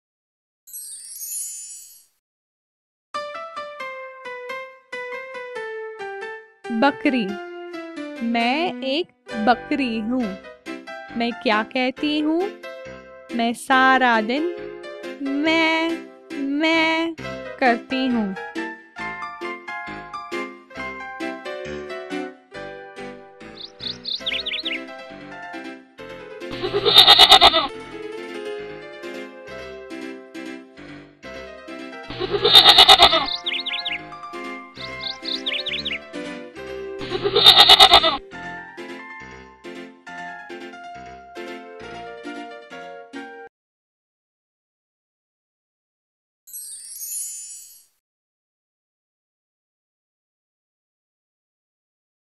Animal Sounds in Hindi - Goat
Goat, I am a Goat, What do I say?
Maa, maa, I say it all day.